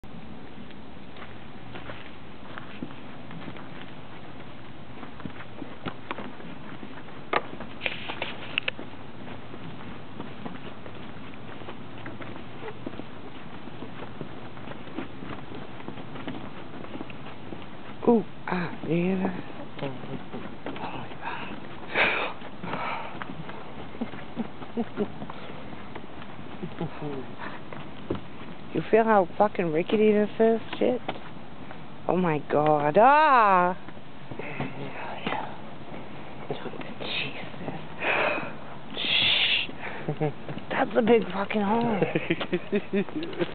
big hole
walking across a bridge in Gorham NH